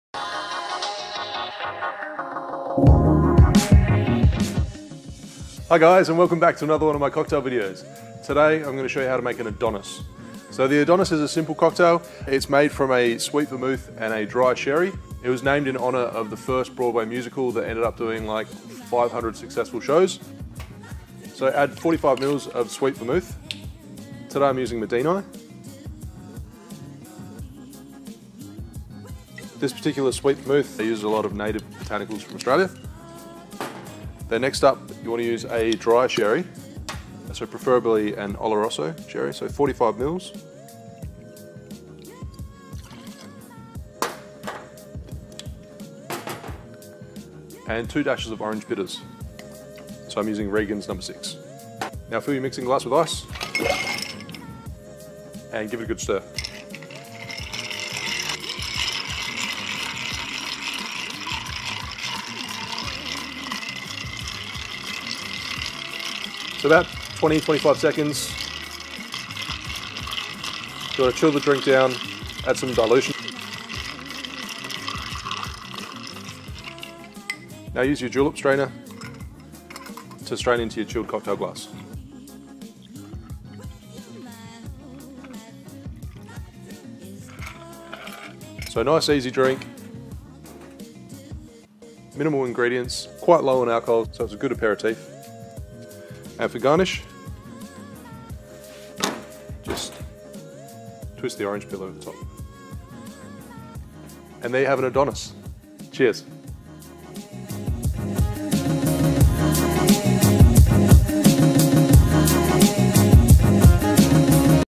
Adonis Cocktail Recipe
How to make an Adonis

The Adonis was first created back in the 1880s and was named in honour of the Broadway musical of the same name. The musical ran for over 500 successful shows. This drink is an ideal aperitif due to its low alcohol content as it contains vermouth & sherry.

INGREDIENTS
- 45mL Sweet Vermouth
- 45mL Olorosso Sherry (Dry)
- 2 Dashes of Orange Bitters

HOW TO
1. Add all ingredients to your mixing glass
2. Stir for approx. 25 seconds
3. Strain into a chilled cocktail glass
4. Garnish with a twist of orange